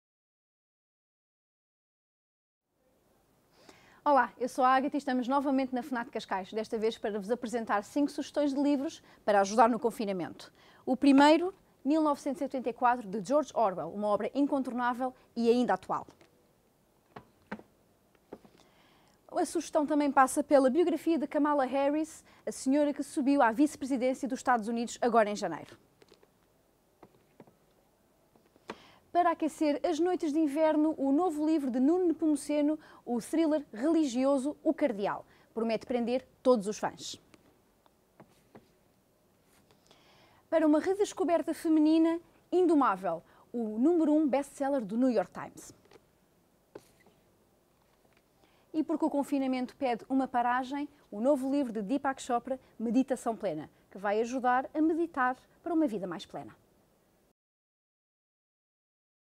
Cultiva-te | Sugestões de Leitura para o confinamento
A Ágata Bettencourt, da FNAC Cascais, tem 5 sugestões de leitura para o confinamento. Podes encontrar estes e outros livros em fnac.pt